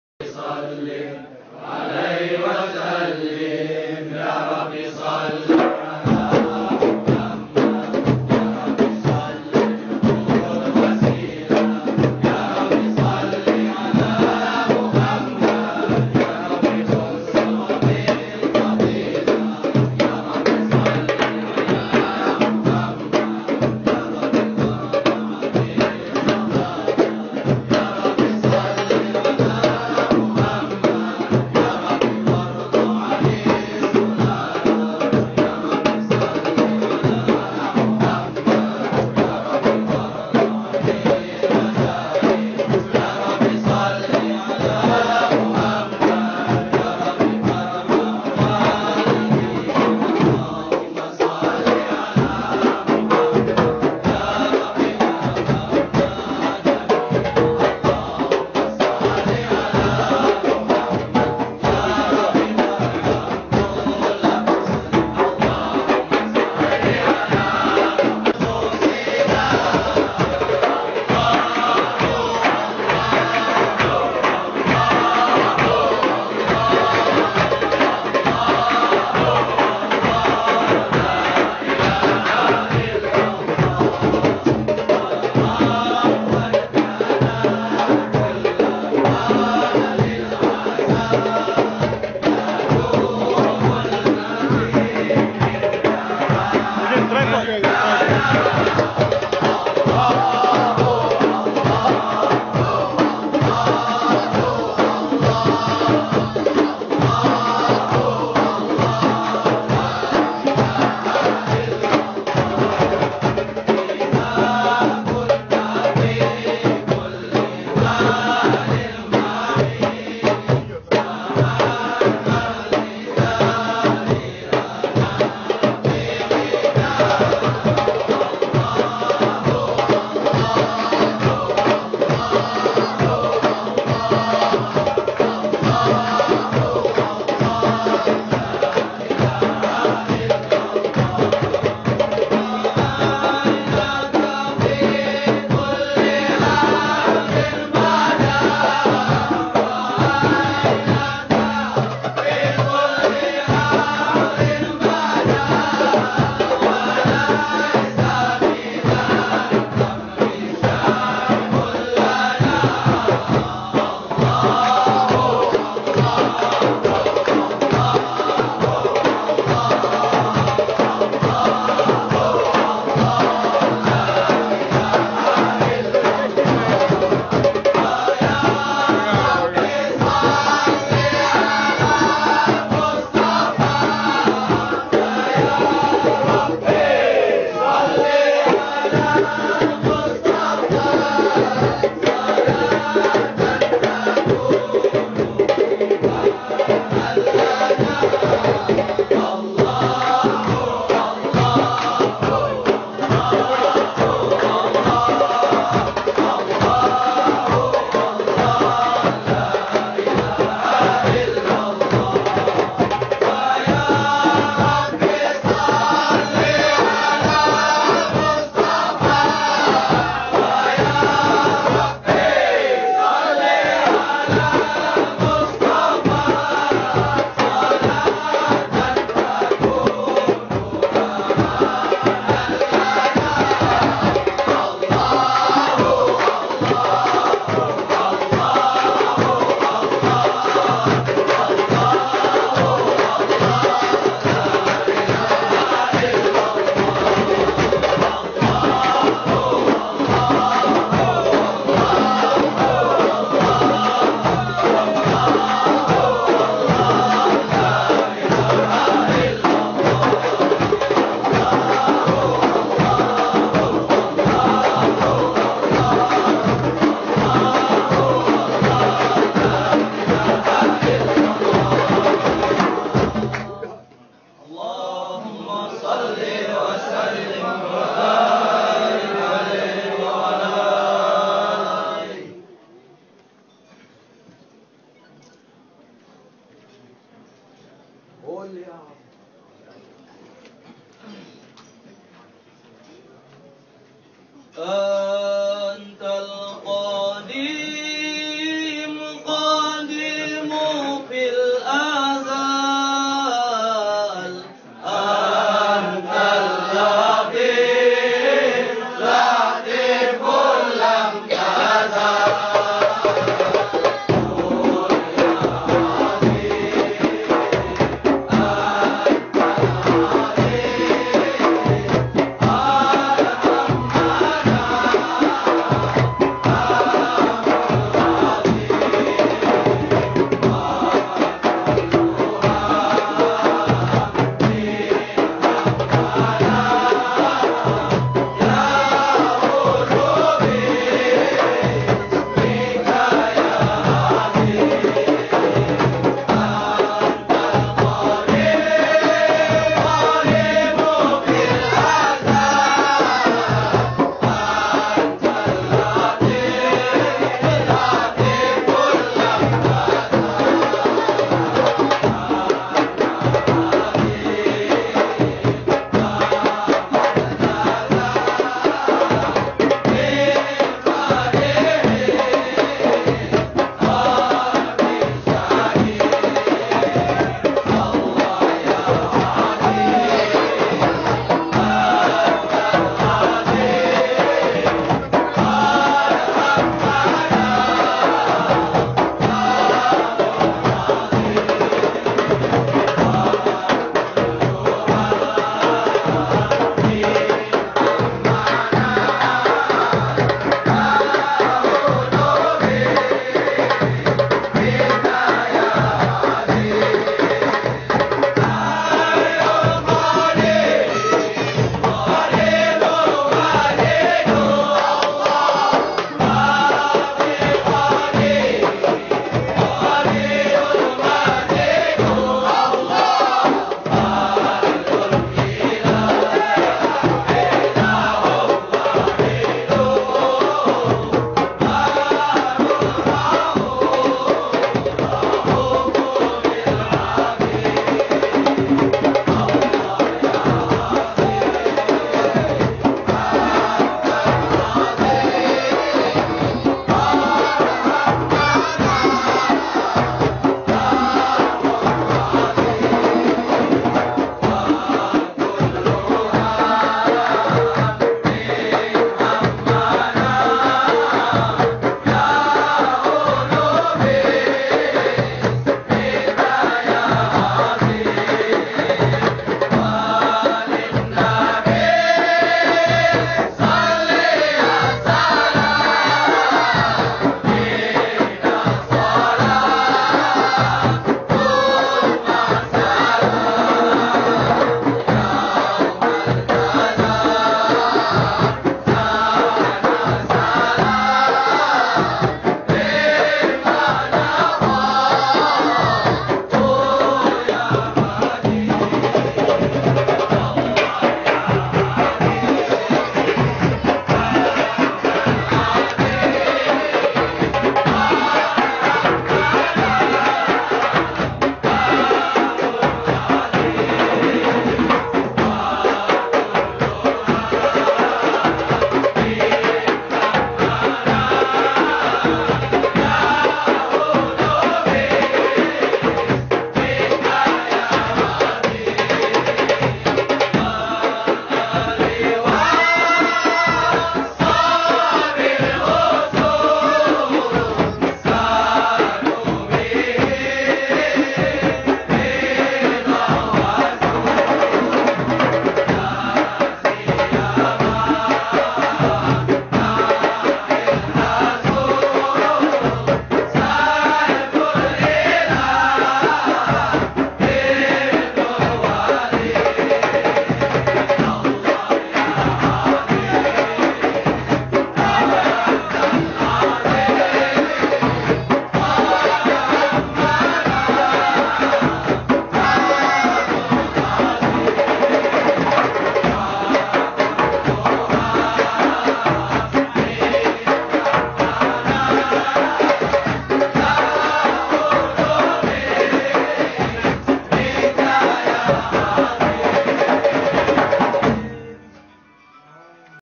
Qasidas after 40th Day Tahlil for HRH Raja Ashman Shah
Mawlana Shaykh Hisham Kabbani
Mawlid un-Nabi  - Kuala Lumpur Malaysia